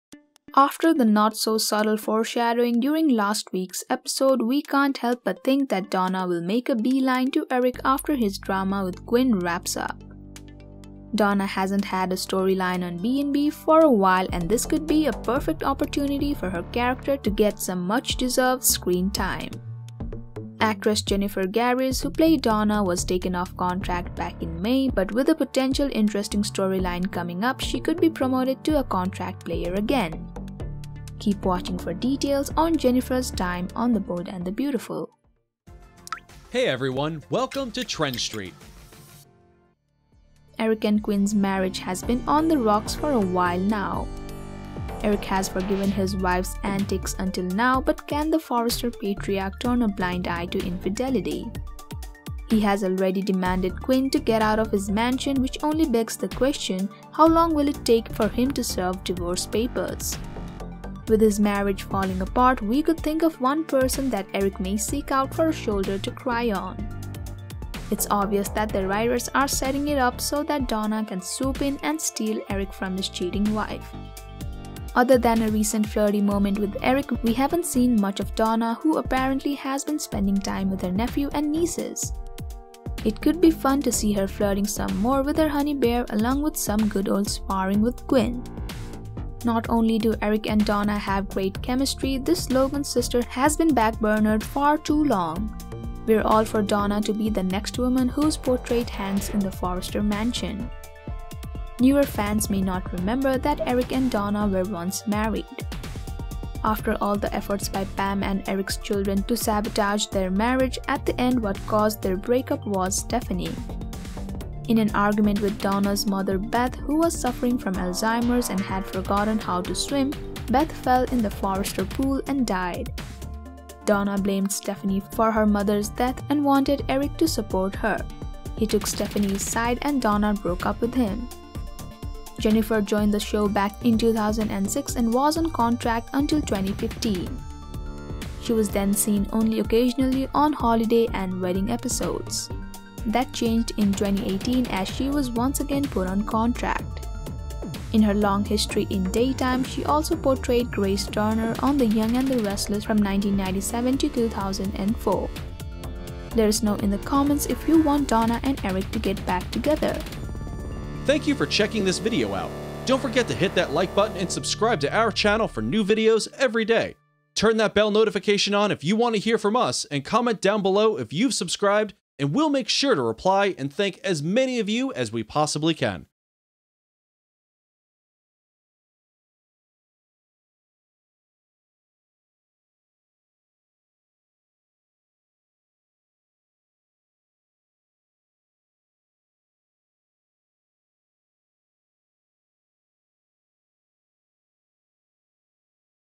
Will Donna and Eric get back together after Quinn’s mistake?
Are Donna and Eric getting back together on the Bold and the Beautiful? After the not so subtle foreshadowing during last week’s episode, we can’t help but think that Donna will make a beeline to Eric after his drama with Quinn wraps up. Keep watching for details on Jennifer’s time on the Bold and the Beautiful.

Here are some of the shows that we love to follow:
General Hospital
Days of Our Lives
Gold Rush
Street Outlaws
Deadliest Catch